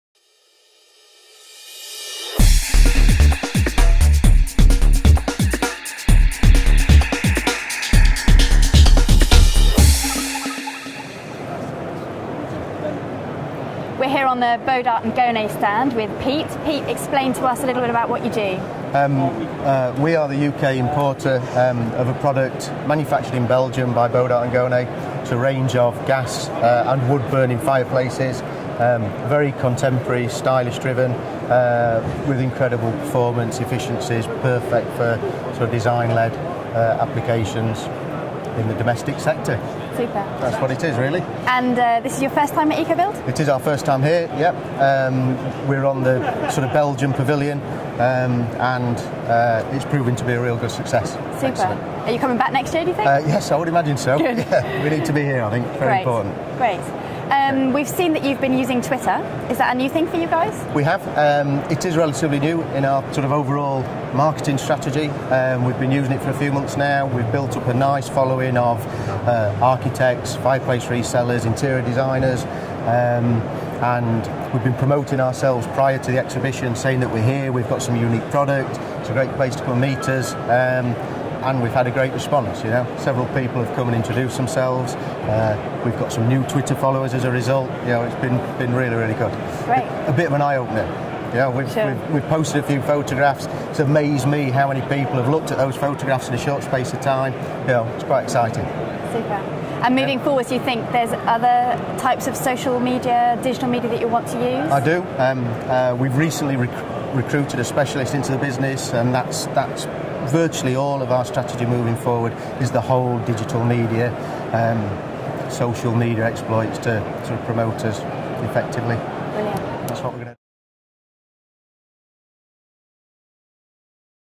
MDiTV - Ecobuild Interview with Pete Lindie, Managing Director (UK Importer of Bodart Gonay)
Pauley Creative caught up with Pete Lindie to find out how Ecobuild 2012 worked for them and how they used Twitter to promote their presence at the show.